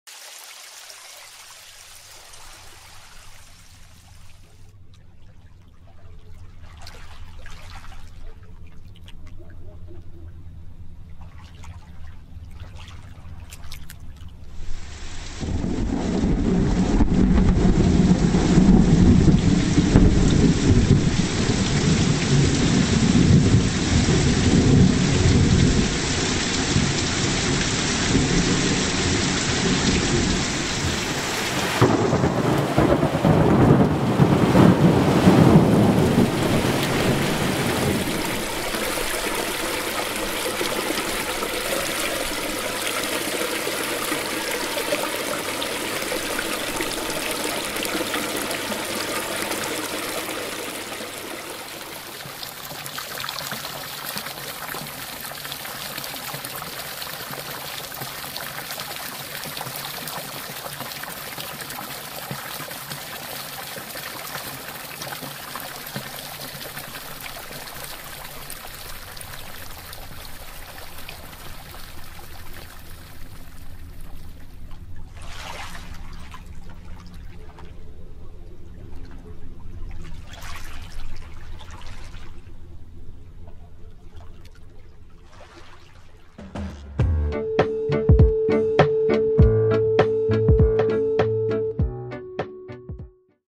Simulation of a Flood wave with Telemac v8p4
Animation created with QGIS and KDENlive.